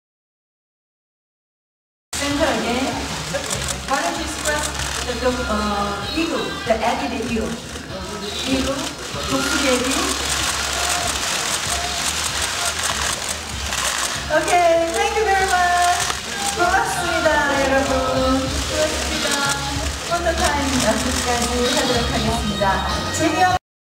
휴 잭맨 내한 기자간담회, (영화 독수리 에디)
아주경제=유대길  기자

배우 휴 잭맨과 덱스터 플레처 감독이 7일 오전 서울 종로구 포시즌스호텔 서울에서 열린 영화 '독수리 에디' 기자간담회에 참석해 포즈를 취하고 있다.

영화 '독수리 에디'는 열정만큼은 금메달급이지만 실력미달 국가대표 '에디'(태런 에저튼)와 비운의 천재코치 '브론슨 피어리'(휴 잭맨)가 펼치는 올림픽을 향한 유쾌한 도전 플레이를 그린 작품이다. '독수리 에디'는 오는 4월 7일 개봉 한다.